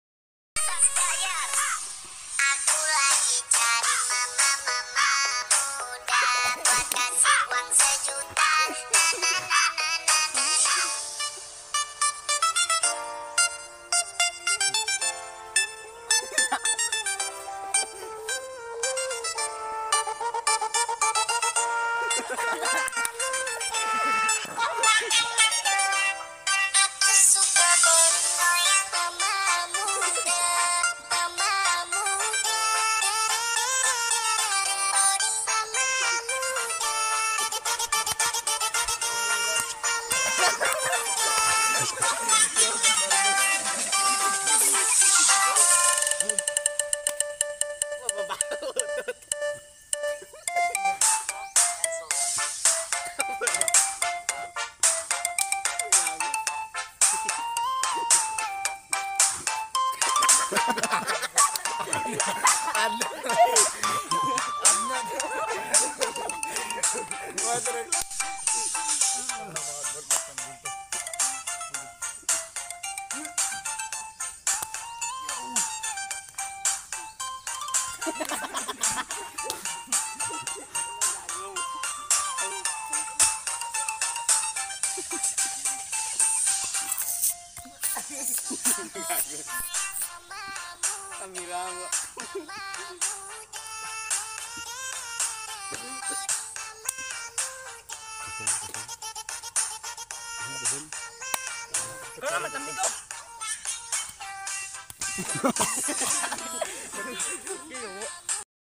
Dance moves and laugh trip
dance moves and laugh trip masaya kmi sa ginagawa namin pampawala ng stress